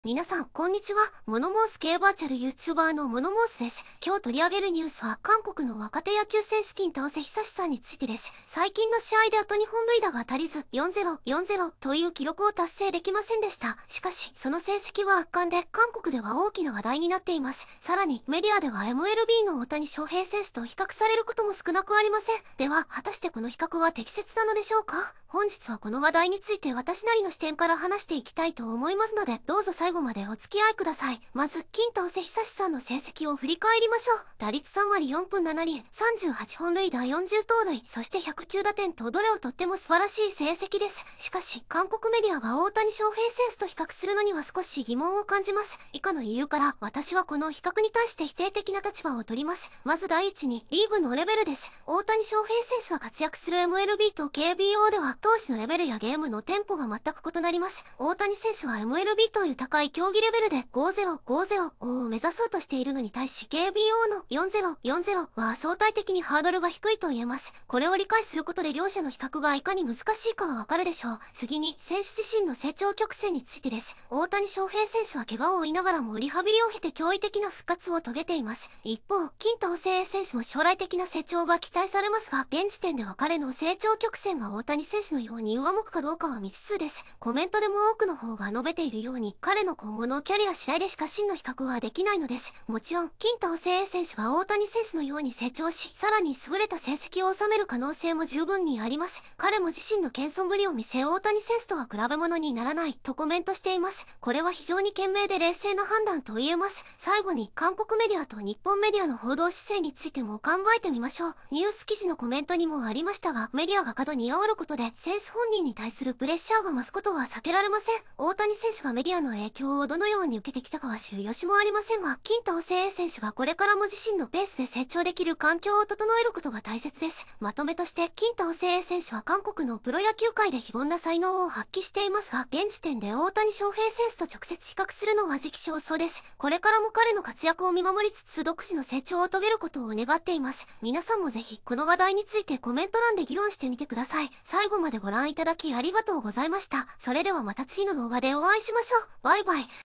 金倒永 vs 大谷翔平：本当にライバル？徹底比較でわかる事実
【出典】
金倒永は「４０―４０」に２本届かず　それでも「大谷翔平のライバルになりつつある」と韓国報道（東スポWEB） - Yahoo!ニュース